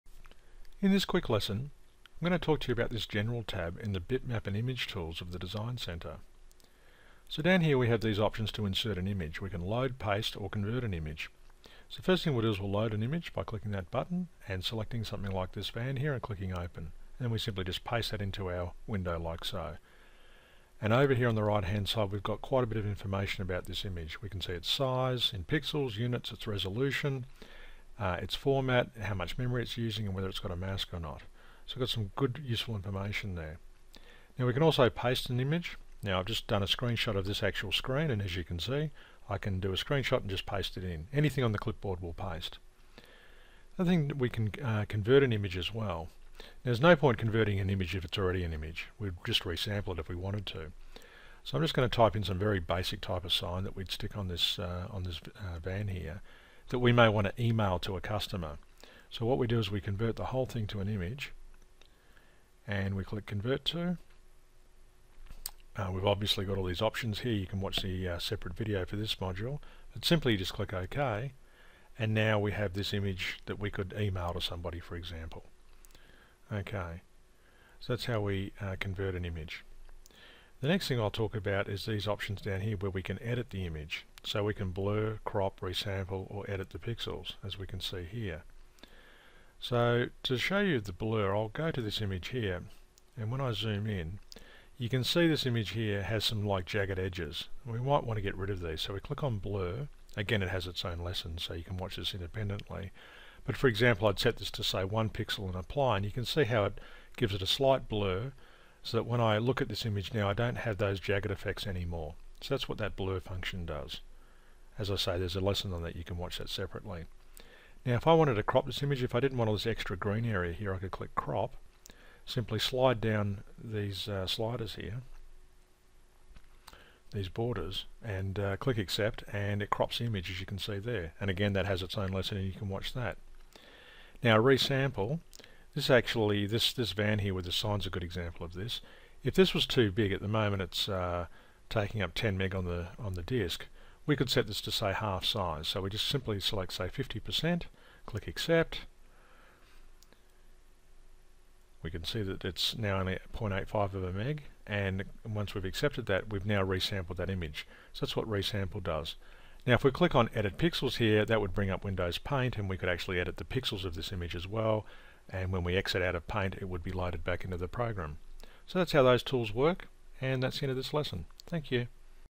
Quick Lesson: DesignCenter - Bitmap and Image Tools
A quick lesson on working with the Image tools from the General tab from the Bitmap and Image Tools, tool panel in the DesignCenter.